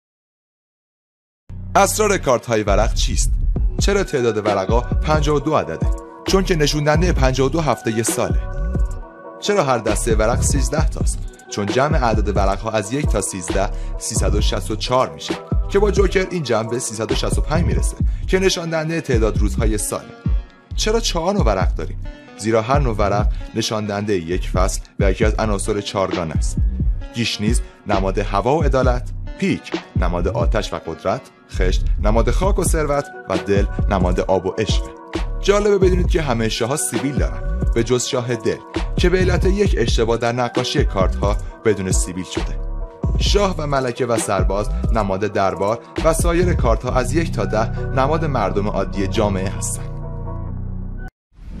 اسرار ورق‌های بازی چیست؟
میدونستید چرا تعداد ورق‌ها ۵۲ عدد است و چه رازی پشت این اعداد نهفته می‌باشد؟